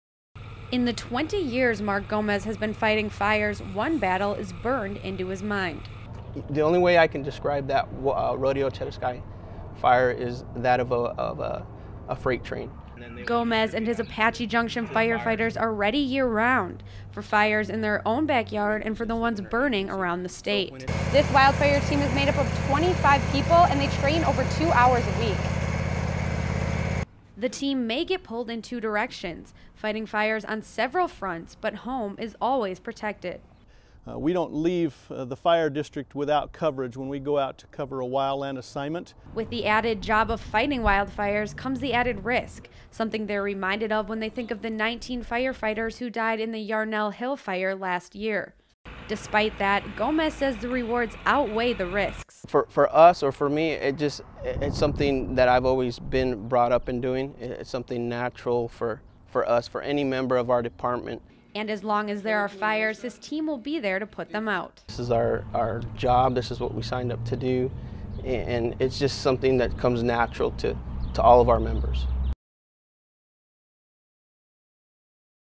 Apache Junction wildfire brigade is always on it's toes
It's been almost a year since the Yarnell fire in Northern Arizona that killed 19 Prescott firefighters. Apache Junction has a crew of firefighters specially trained for wildfires close to home that is ready for action throughout the entire state. Cronkite News reporter Lauren Wells has the story.